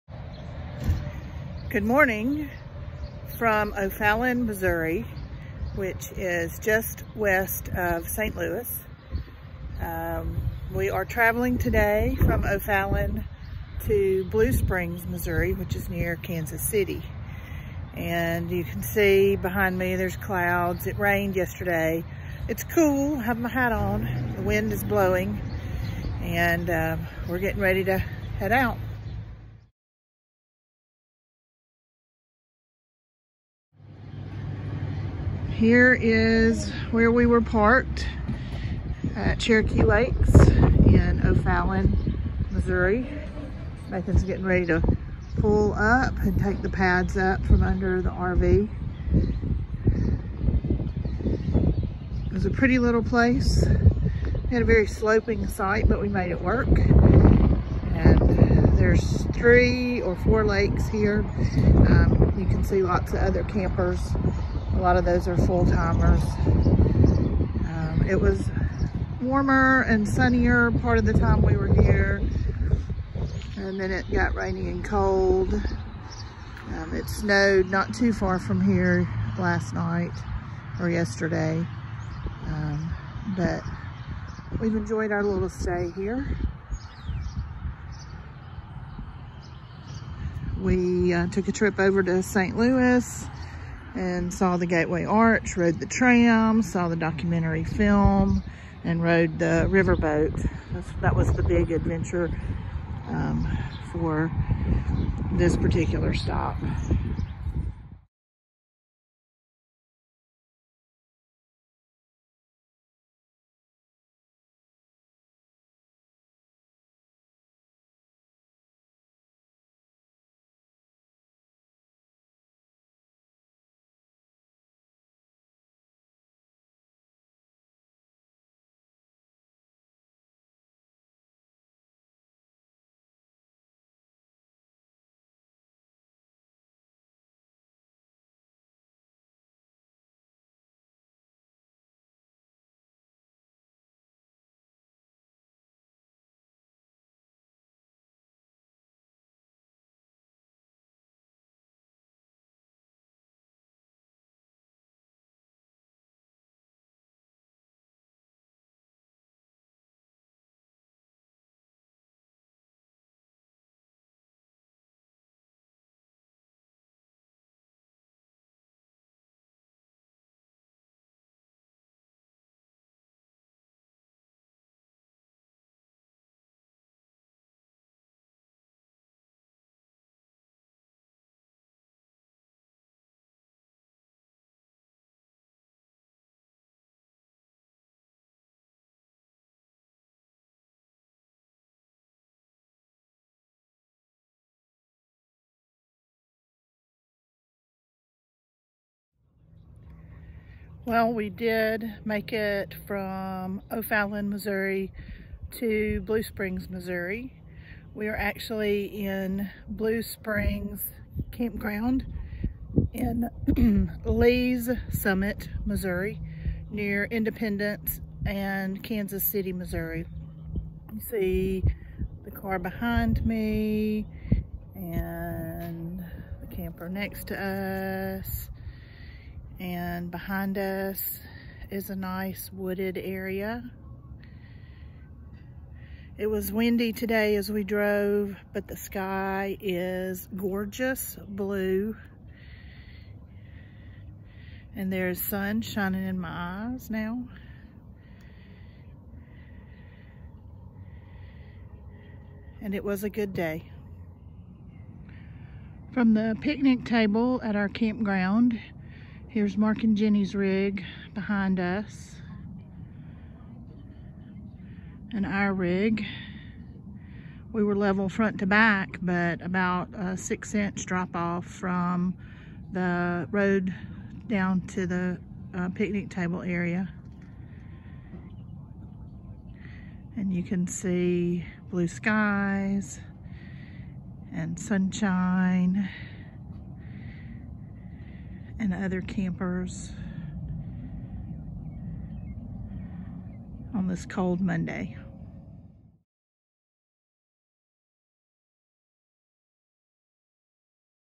Stop 4, Gateway Arch, Stop 5
Leaving O’Fallon, MO on April, 18, 2022. Photos of our trip to the Gateway Arch on April 15, 2022.
Our 5th stop in Lee’s Summit, MO on April, 18, 2022.
The picture of Beth and the Buffalo is a recreation of sorts of a photo from several years ago of son Jamie with a Buffalo in a museum in Oklahoma